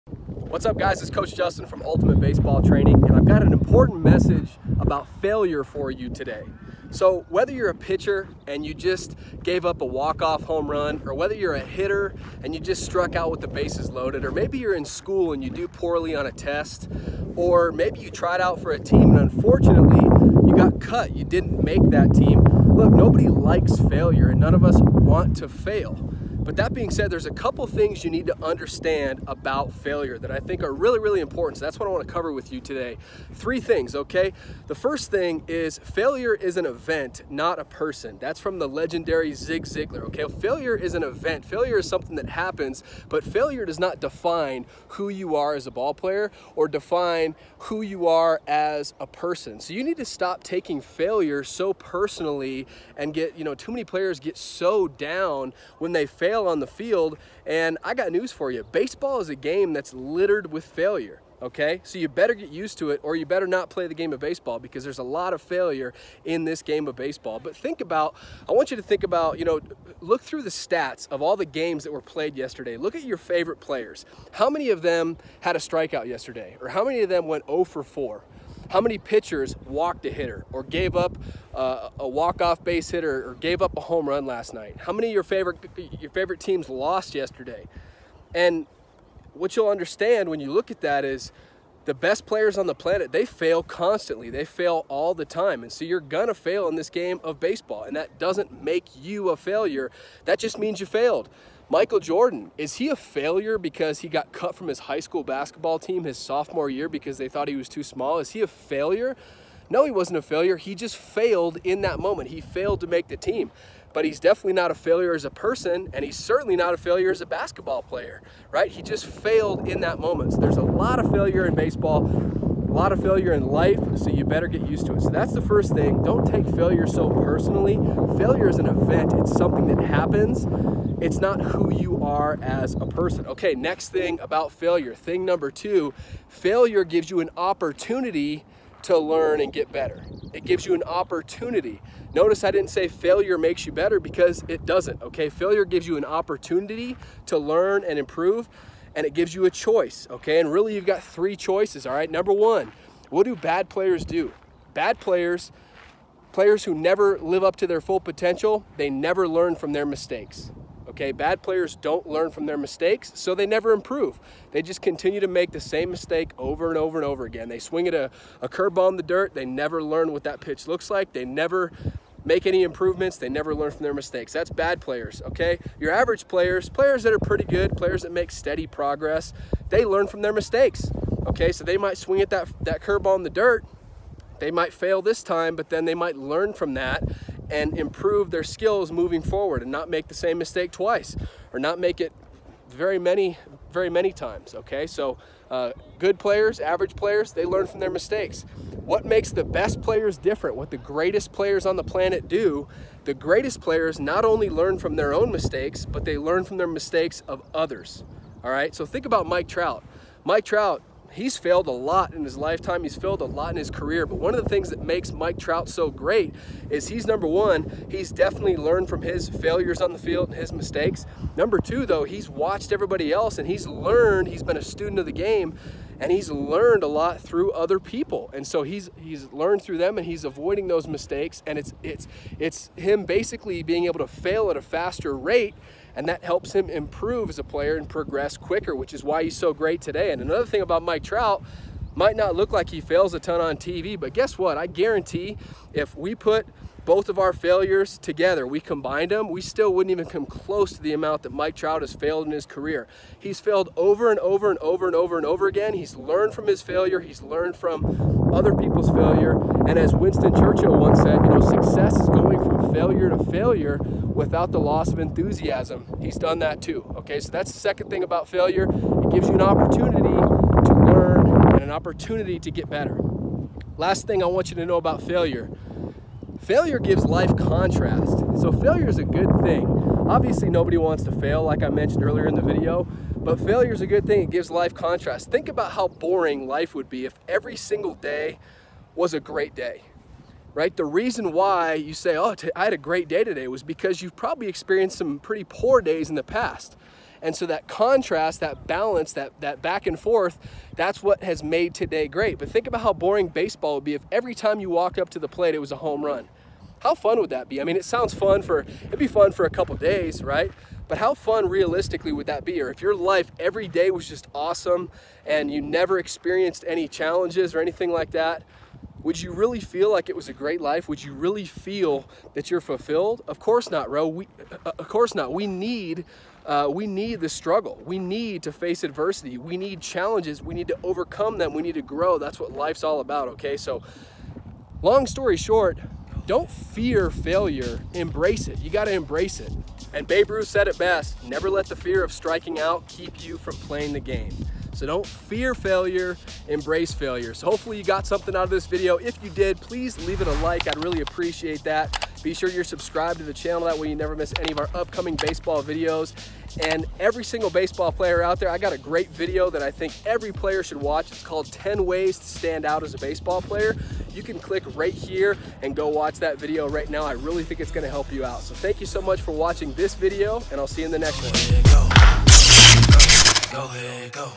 Don’t Fear Failure!
In this video, Coach Justin from Ultimate Baseball Training talks about failure and why you should never fear failure. Failure is an event, not a person. It gives you the opportunity to learn and get better and it gives your life contrast. Instead of fearing failure, embrace it and learn from your mistakes.